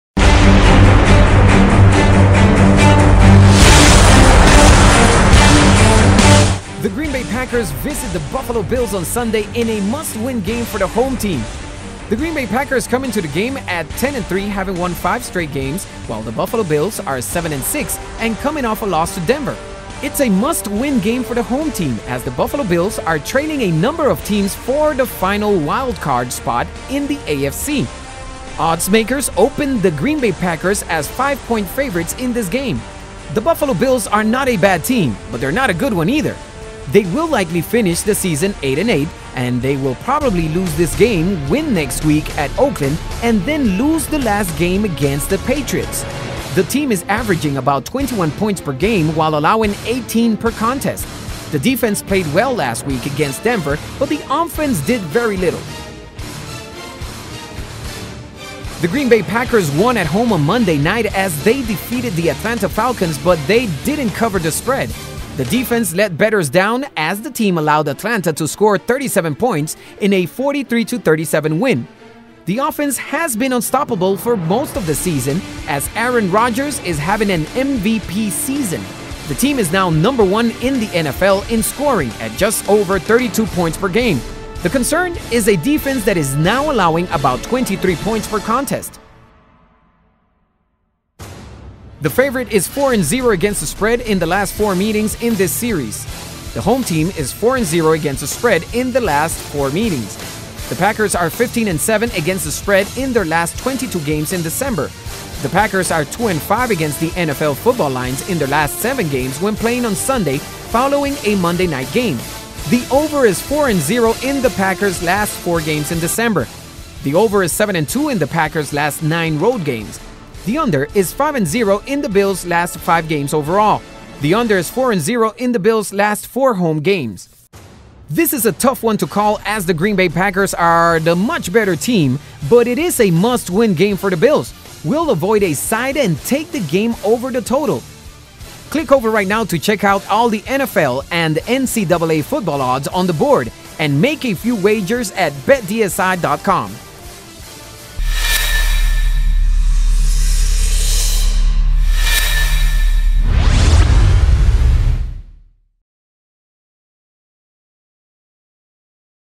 Green Bay Packers vs Buffalo Bills Odds NFL Picks and Predictions
Game Trends

The favorite is 4-0 against the spread in the last 4 meetings in this series. The home team is 4-0 ATS in the last 4 meetings. The Packers are 15-7 ATS in their last 22 games in December. The Packers are 2-5 against the NFL football lines in their last 7 games when playing on Sunday following a Monday night game. The Over is 4-0 in the Packers last 4 games in December. The Over is 7-2 in the Packers last 9 road games. The Under is 5-0 in the Bills last 5 games overall. The Under is 4-0 in the Bills last 4 home games.

NFL Football Picks

This is a tough one to call as the Green Bay Packers are the much better team but it is a must-win game for the Bills. I will avoid a side and take the game over the total.